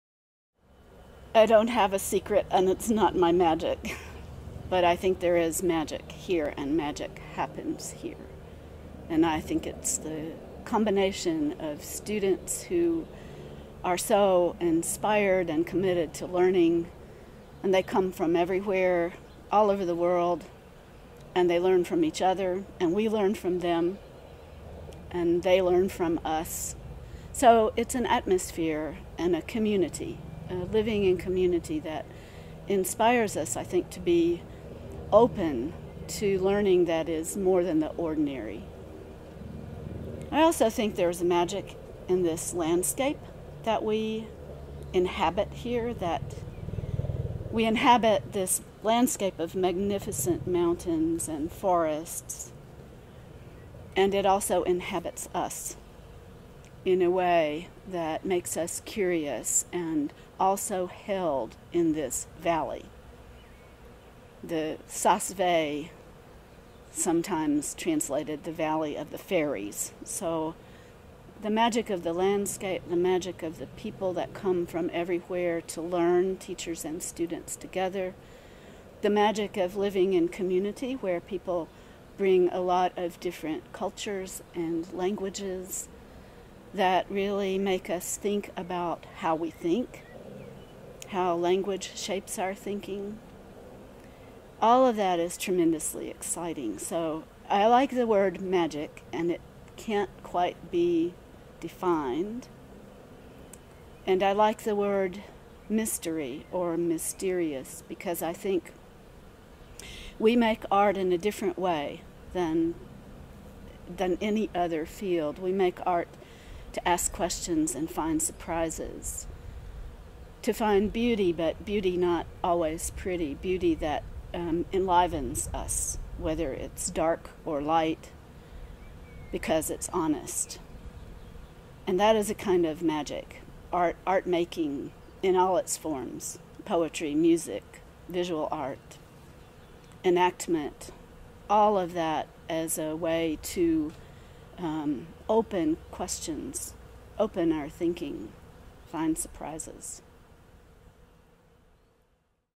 Sally Atkins. We make Art to ask Questions and find Surprises. 2016
Sally Atkins, Professor at the AHS (Arts, Health and Society) Division from The European Graduate School / EGS. Saas Fee, Switzerland 2016.

We have asked EGS core faculty member Professor Sally Atkins: „Over all these years you are such a successful teacher. The student evaluations from your seminars are always off the charts. What’s your magic, what’s your secret?“